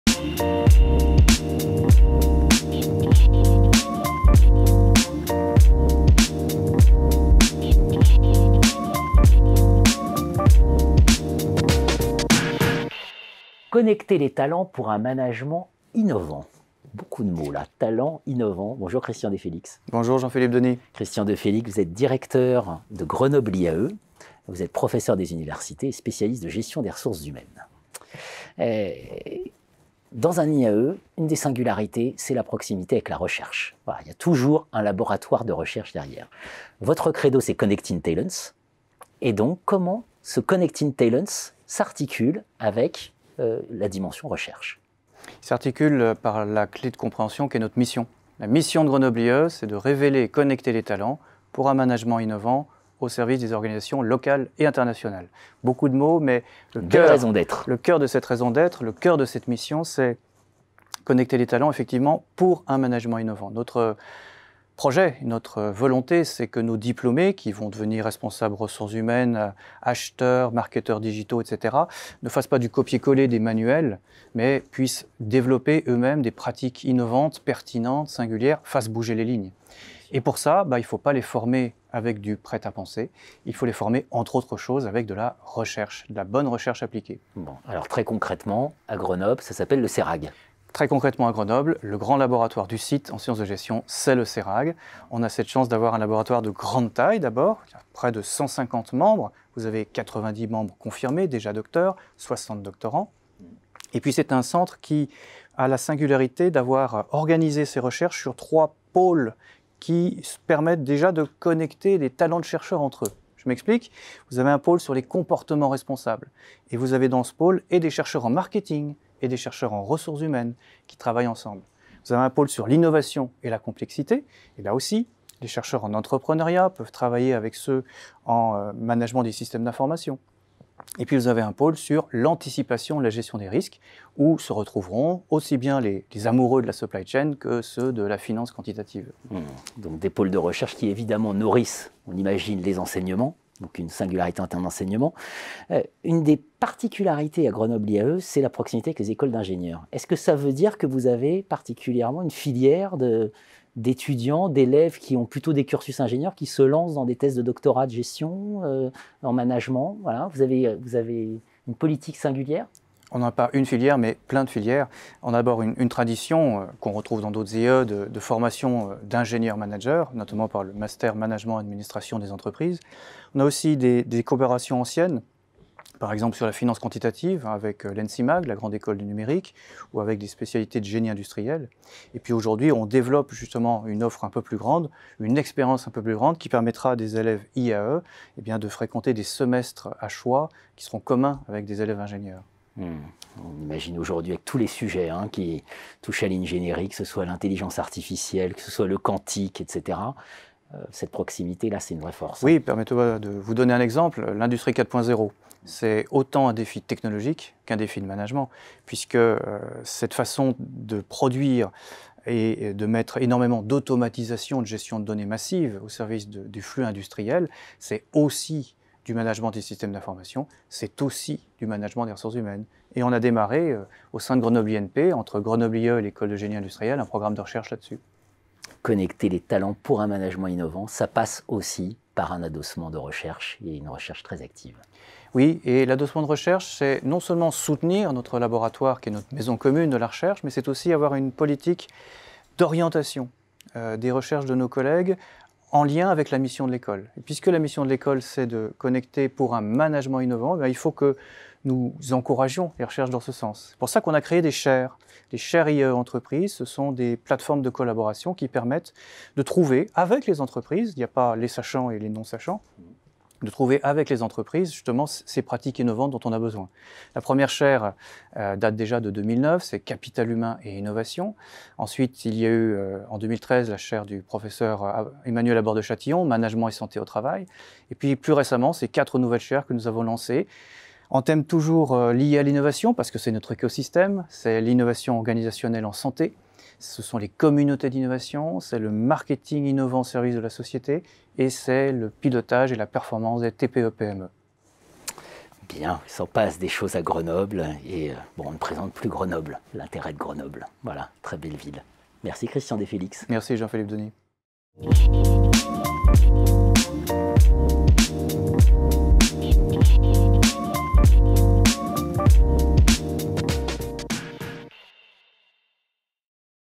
Rapprocher les écoles universitaires de management et les écoles d’ingénieurs [C. Defélix]
Xerfi Canal a reçu Christian Defélix, Directeur, Grenoble IAE, IAE France, pour parler des écoles universitaires de management et des écoles d'ingénieurs.